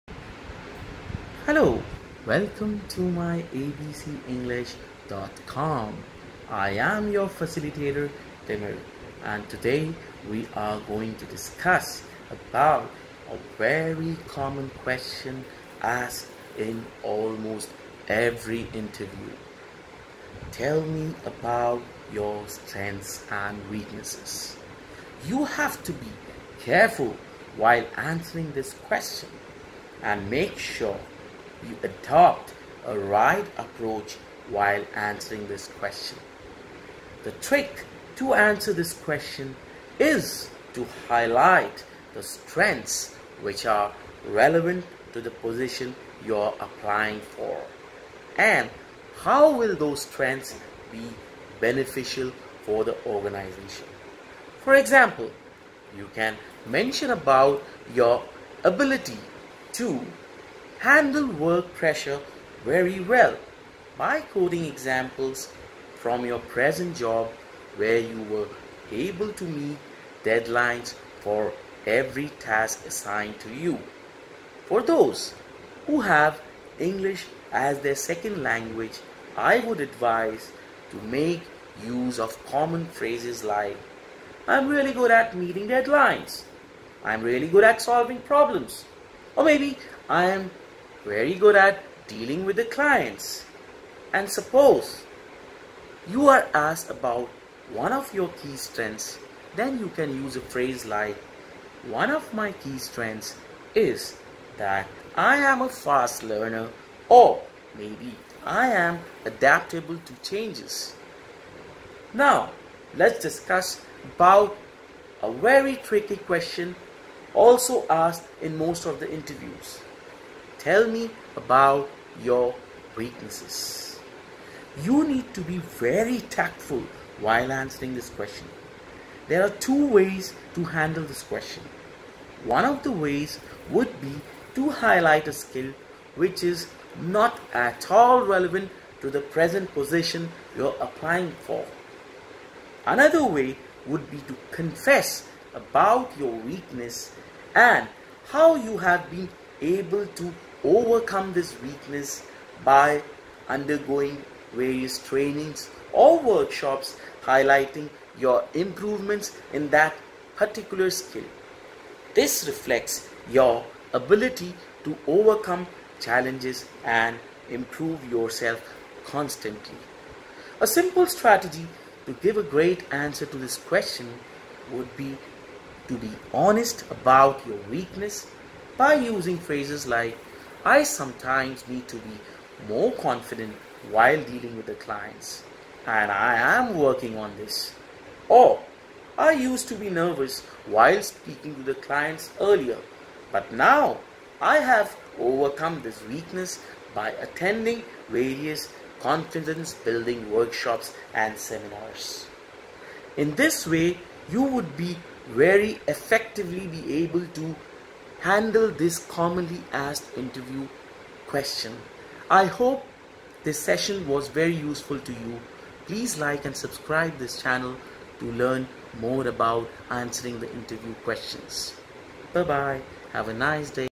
Tell me about your strengths and weaknesses | Timir Naha
This video by Timir Naha talks about a common interview question Tell me about your strengths and weaknesses or What are your strengths and weaknesses?
Here is a link to another useful interview video